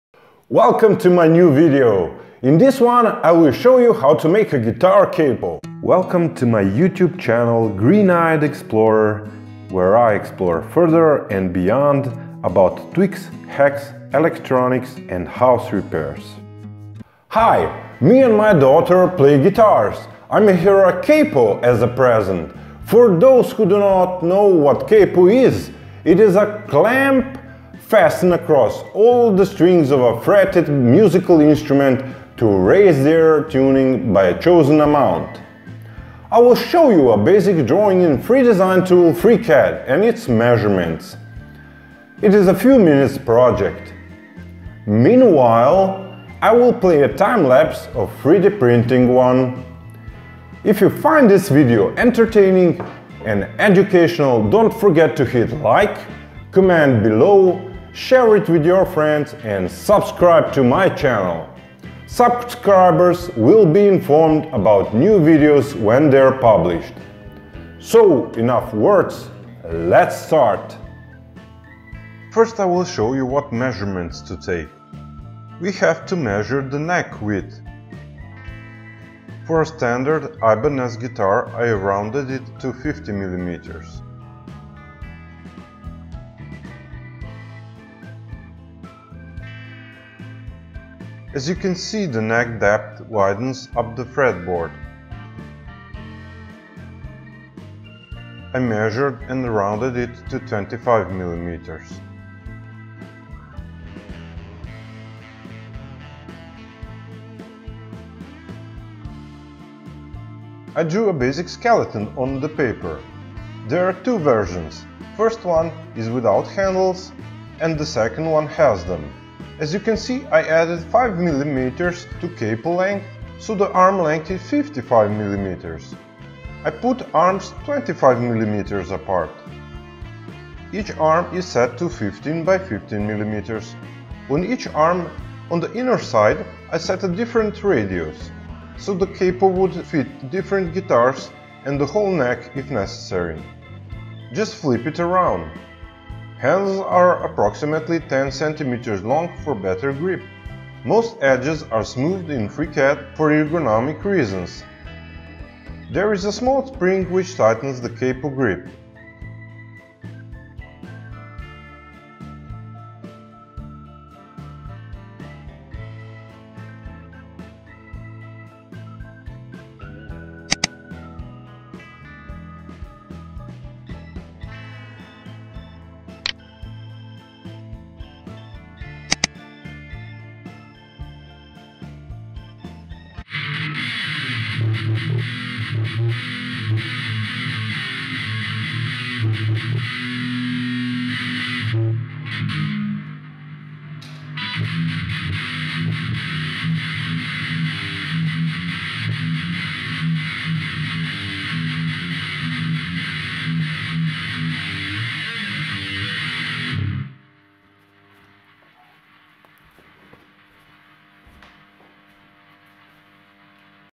🔴 GUITAR CAPO DIY 🎸 3D PRINT
We will see how to do a simple design of a guitar capo with an open-source tool FreeCAD. I will show you a basic drawing and measurements. It is a few minutes project. Meanwhile I will play a time-lapse of 3D printing one. STL files can be found on my homepage or on Thingiverse.com.
The capo is high quality, assembly is fluent. You only need a spring and a M4 screw with a nut.
It's a FreeCAD tutorial / FreeCAD tutorials for beginners.

Do you like my work?
1. Hit LIKE.
4. SHARE.

Spring for the guitar capo (The one I use. 1mm wire thickness, outer dia 7mm, 20mm length.)